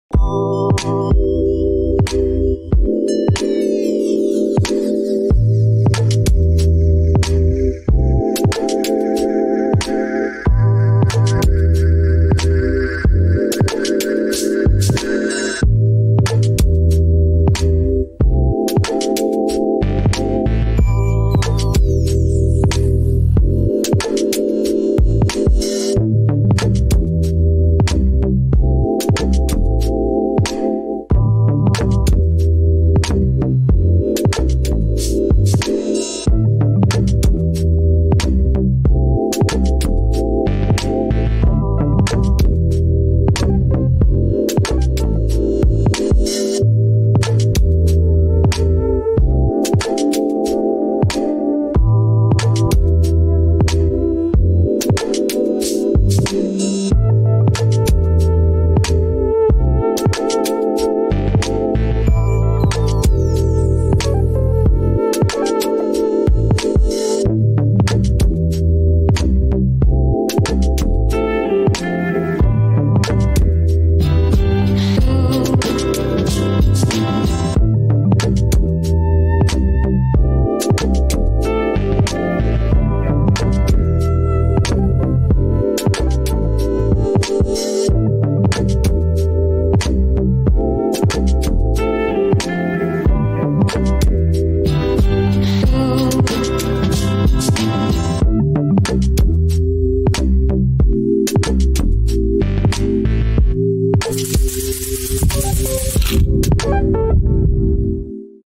Why the Proboscis Monkey Has a Big Nose
The bigger the nose, the more attracting they are to females.